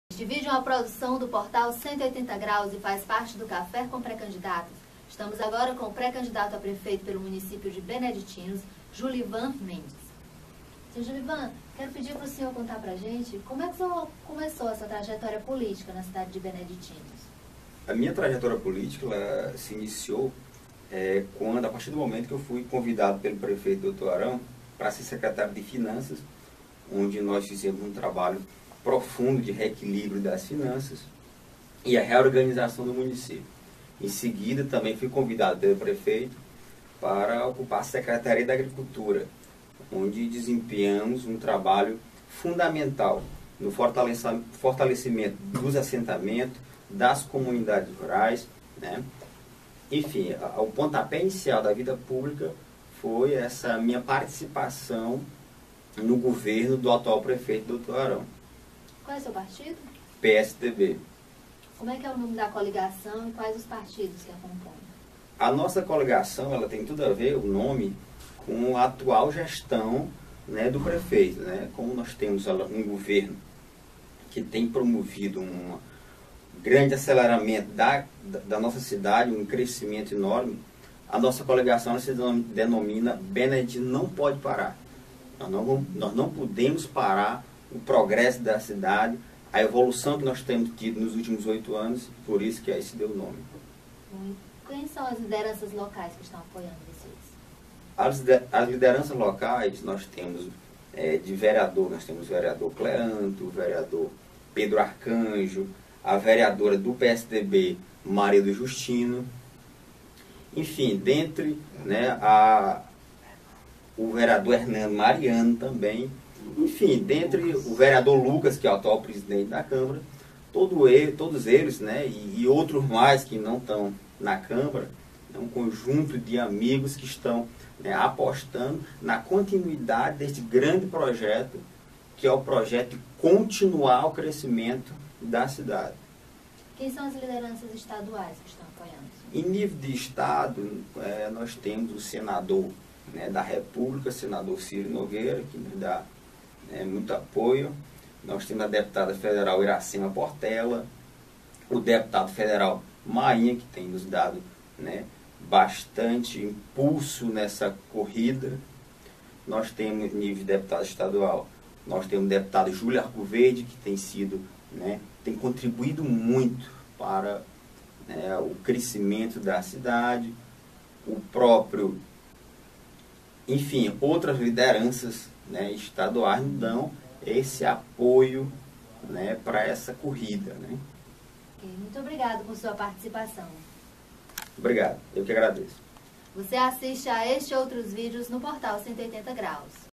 Café com pré-candidatos: Jullyvan Mendes concedeu entrevista ao 180
Jullyvan Mendes (PSDB) esteve na sede do portal 180, nesta quinta-feira (09/08) e participou do Café com Pré-candidatos. Confira sua entrevista.